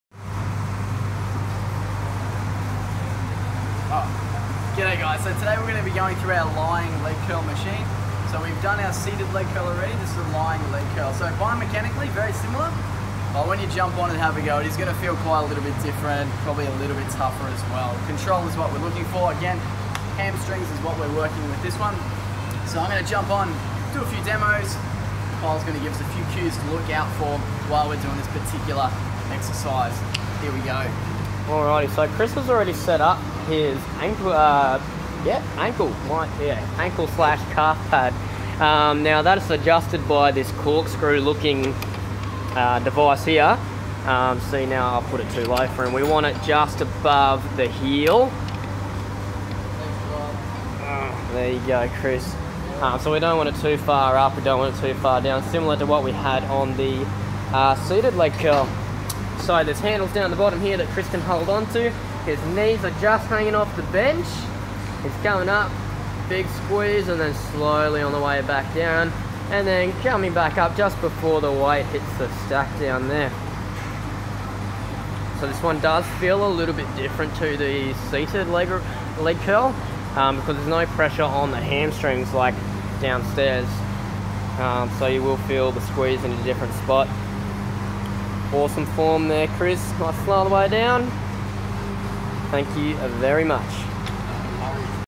Lying Leg Curl
Set up and technique for the Lying Leg Curl on machines.